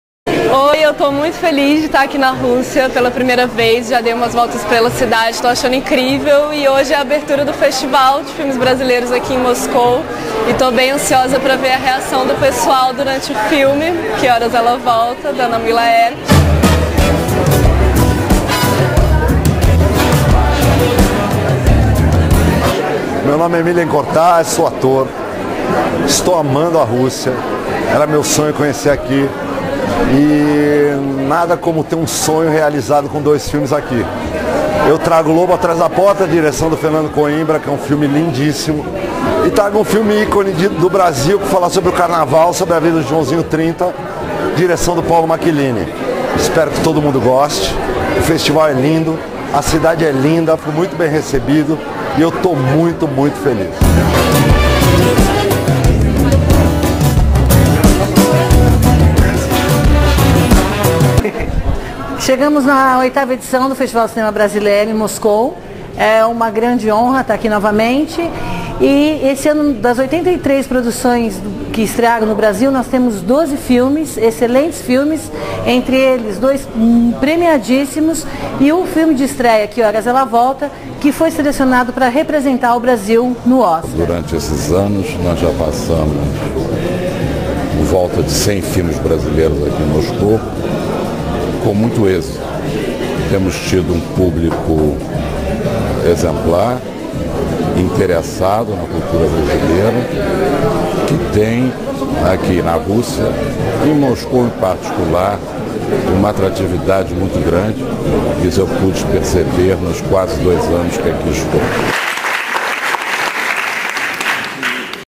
VIII Festival de Cinema Brasileiro de Moscou
O VIII Festival de Cinema Brasileiro de Moscou foi aberto no dia 27/10/2015, com a exibição do filme “Que Horas Ela Volta?”, de Anna Muylaert. O evento teve a participação especial da atriz  Camila Márdila e do ator Milhem Cortaz. O Embaixador Antonio Guerreiro fez o discurso de abertura do festival, realizado no tradicional cinema 35mm.  Organizado pela Linhas Produções Culturais e pela Embaixada do Brasil, o evento já se tornou parte do calendário cultural de Moscou. Desde 2008, o festival exibiu mais de 100 filmes brasileiros na Rússia.